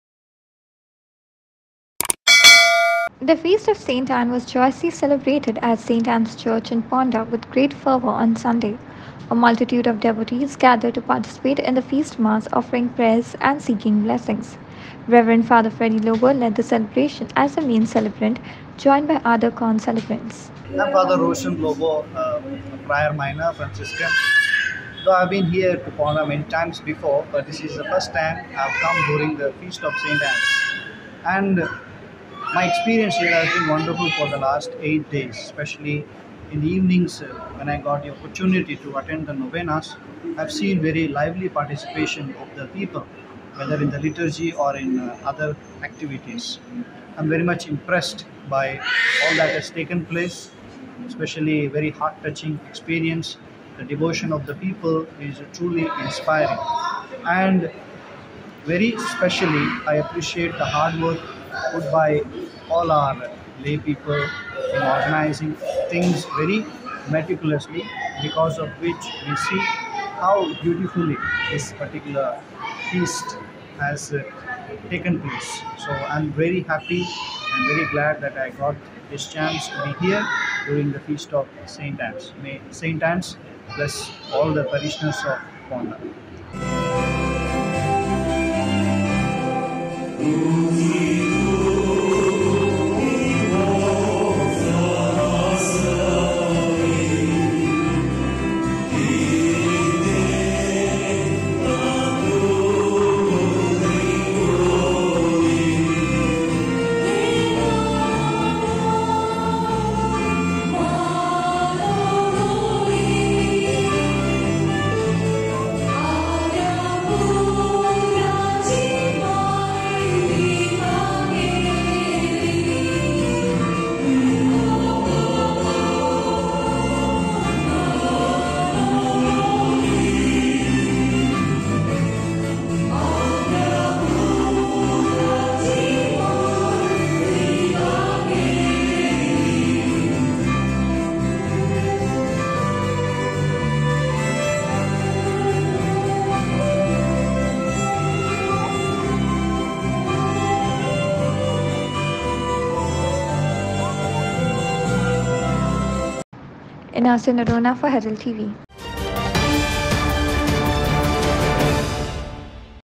Feast of St. Anne celebrated at St. Anne's Church, Ponda on Sunday
The feast of St. Anne was joyously celebrated at St. Anne's Church, Ponda with great fervor on Sunday. A multitude of devotees gathered to participate in the feast mass, offering heartfelt prayers and seeking blessings. Reverend Fr. Freddy Lobo led the celebration as the main celebrant, joined by other con-celebrants.